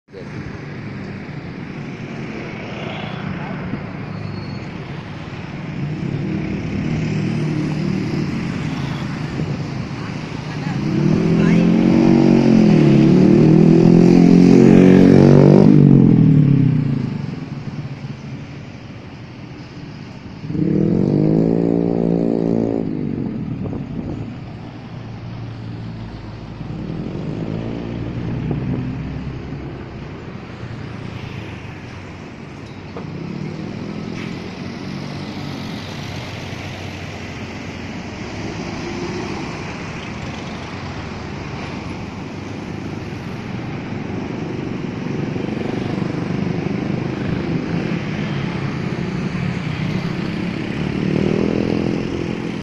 รถไฟฟ้า ของเด็กๆ
มาให้ชมกัน ราคาคันละ 3,750 บาท
เหมือนมากๆ ข้างๆ วินมอไซด์